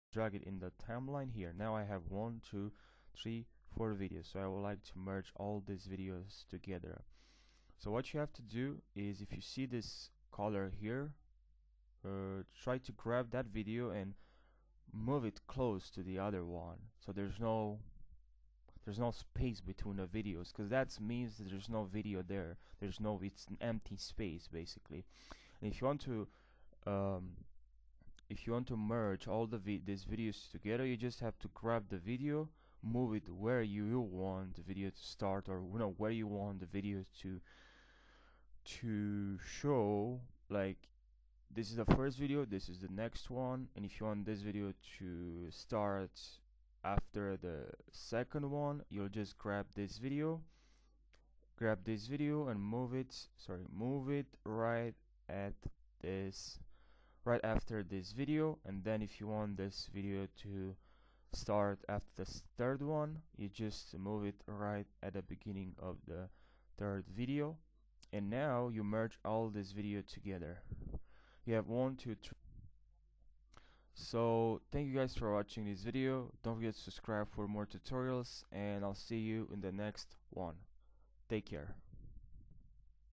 Kdenlive Tutorial: How To Combine Clips In Kdenlive
You won't want to miss a single detail of this tutorial since I will take you step-by-step through the entire procedure. So please follow along with me and watch the entire video on How To Merge Clips In Kdenlive

Did this how-to video assist you in finding a solution to the issue you were having? If you have a friend who wants to learn how to do this, send them this on how to combine clips in kdenlive

Timestamp:
00:00 Intro
00:34 Start